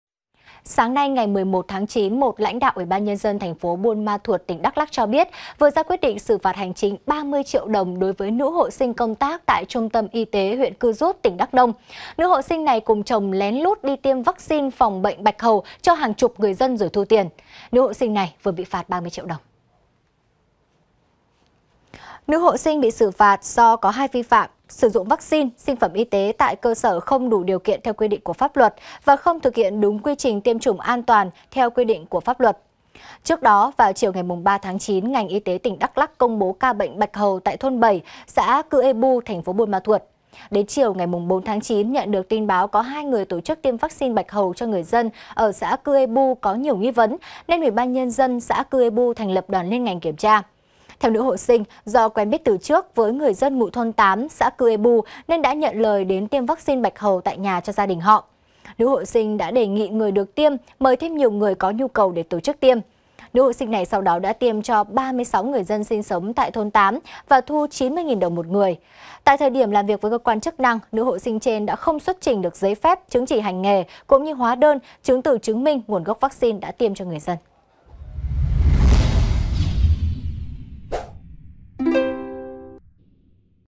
Nữ hộ sinh lén lút tiêm vắc-xin bạch hầu bị phạt 30 triệu đồng| VTC14
VTC14 |Sáng nay, một lãnh đạo UBND TP. Buôn Ma Thuột, tỉnh Đắk Lắk cho biết, vừa ra quyết định xử phạt hành chính 30 triệu đồng đối với nữ hộ sinh công tác tại Trung tâm Y tế huyện Cư Jút, tỉnh Đắk Nông. Nữ hộ sinh này cùng chồng lén lút đi tiêm vắc-xin phòng bệnh bạch hầu cho hàng chục người dân rồi thu tiền. Nữ hộ sinh này vừa bị phạt 30 triệu đồng.